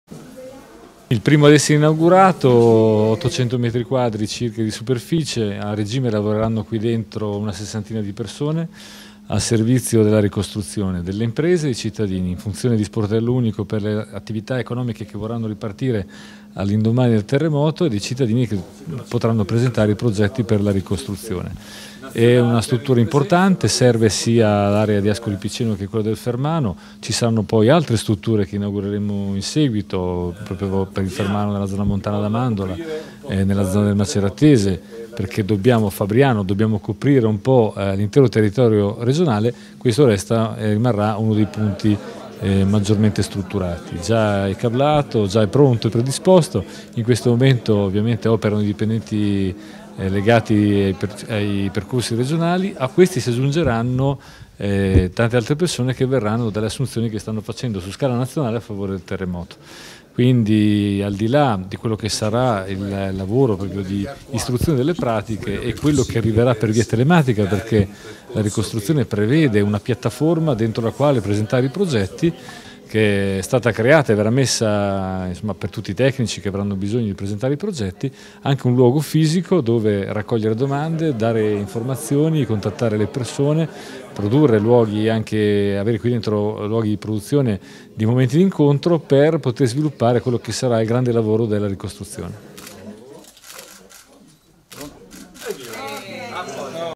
Ascoli Piceno: inaugurata la prima sede dell'Ufficio Speciale per la ricostruzione post sisma
Ceriscioli: "A disposizione delle imprese e cittadini"